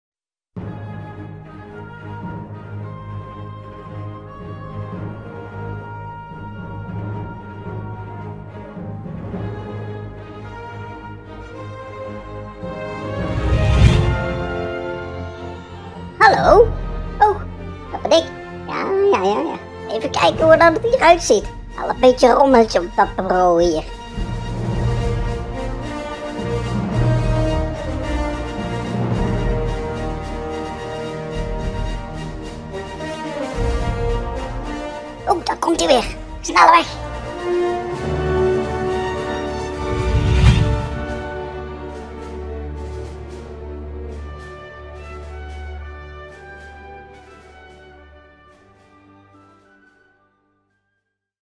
Workshop - Making of Het blauwe manneke Mooky
Achter de schermen bij het maken van het 3D blauwe manneke  Mooky voor de Workshop "Speelfilm". op BS. Markeent